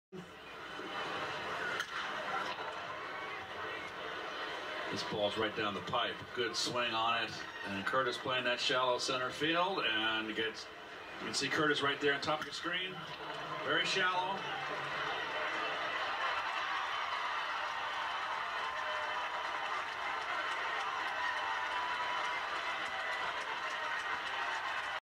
Harrison Bader Triple
St Louis Cardinals Harrison Bader cracks a 3-run triple to centerfield against the Mets on 3/5/17.